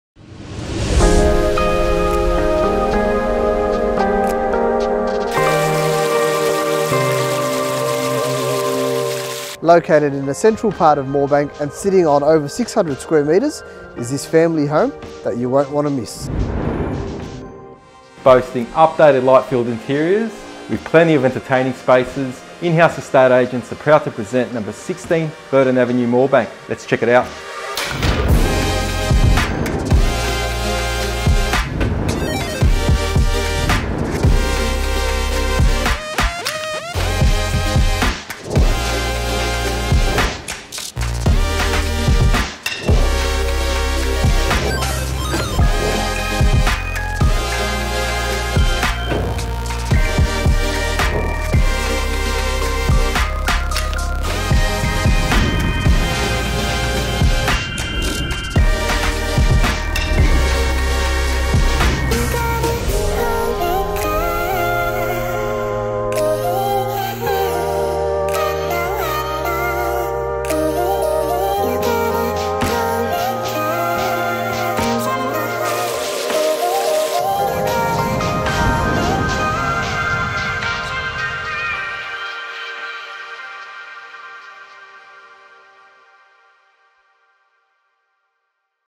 16 Burton Avenue, Moorebank
Instantly appealing and conveniently positioned only moments to Moorebank Shopping Village, a superb opportunity awaits. This immaculately presented family residence delivers tastefully updated light filled interiors, a floorplan perfectly configured for family living with an easy transition to an abundance of outdoor entertaining space for family and friends to enjoy.